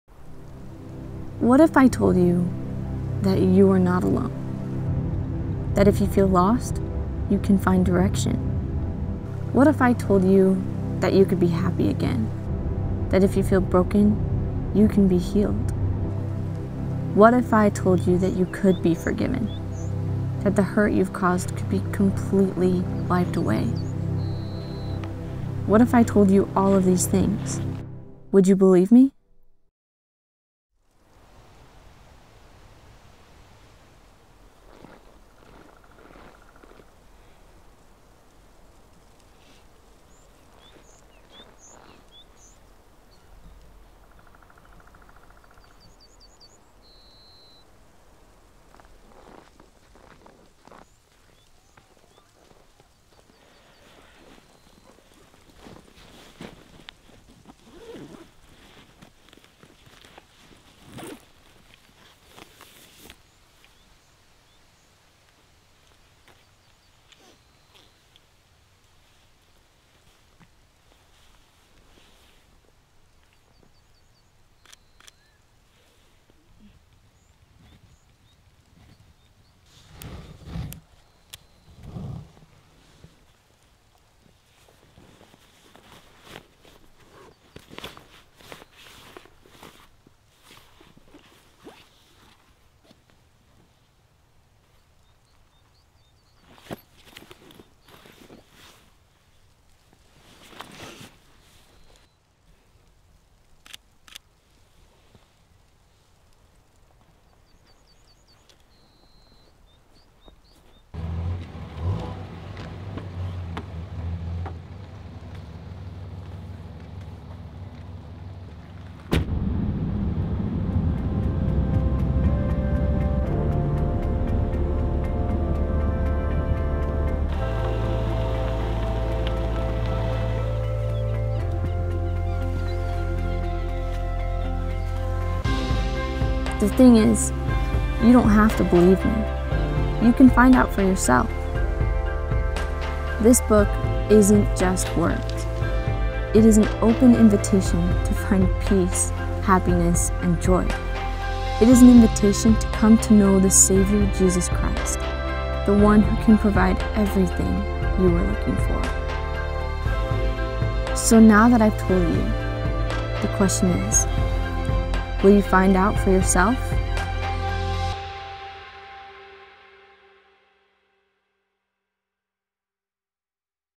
What If I Told You? | Cinematic a7siii Short Film
This book means the world to me, and I made this video to share the impact it has had on my life. Please if you have questions, don't hesitate to reach out!

Gear Used:
Sony a7siii
Sigma 24-70mm f2.8
Sony G-Master 70-200mm f2.8
DJI RS2